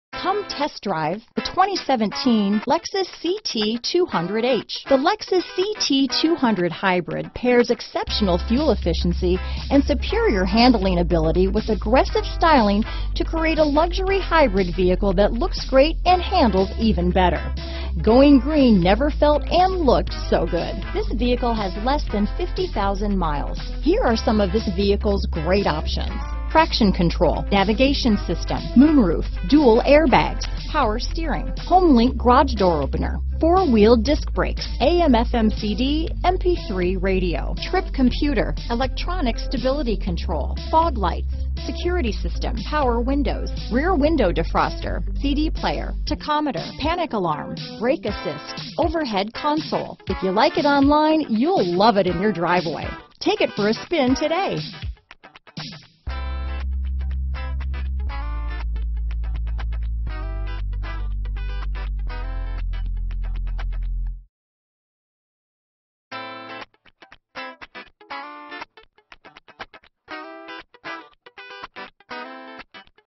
2017 Lexus CT Oxnard, Ventura, Camarillo, Thousand Oaks, Simi Valley, CA LXP2611
Redline Used 2017 Lexus CT available in Oxnard, California at DCH Lexus of Oxnard. Servicing the Ventura, Camarillo, Thousand Oaks, Simi Valley, CA area.

No payments for 90 Days - $0 Down delivers vehicle for most qualified buyers. Aluminum Door Sill Scuff Plates, Aluminum Pedals, Electrochromic Auto-Dimming Rear-View Mirror, F Sport Package w/NuLuxe Interior, F Sport Rear Spoiler, F Sport-Tuned Suspension, HomeLink Universal Transceiver, Leather-Trimmed F Sport Steering Wheel, Navigation System, Perforated Leather-Trimmed Shift Knob, Premium Package for Navigation, Rain-Sensing Variable Intermittent Wipers, Seat Comfort Package, Tilt & Slide Moonroof, Wheels: 17 x 7.0 Two-Tone Machined. CARFAX One-Owner. Clean CARFAX. Obsidian 2017 Lexus CT 200h 43/40 City/Highway MPG Continuously Variable (ECVT) 43/40 City/Highway MPGbrAwards:br  * 2017 KBB.com Brand Image Awards